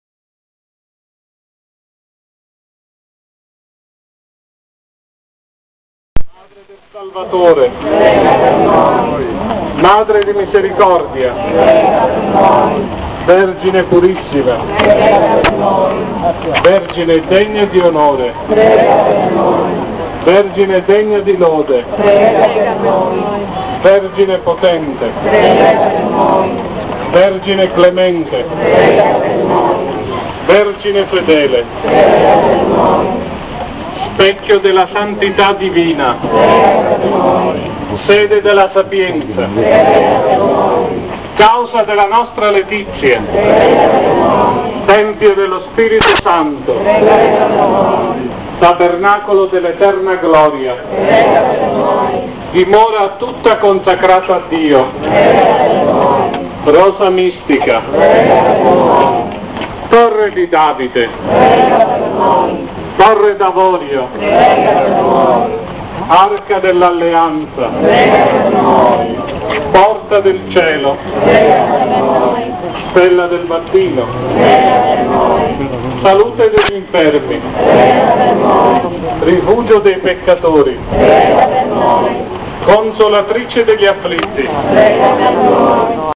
maratea la festa di fiumicello 2017 la processione parte 2
video di vittorio zaccaro maratea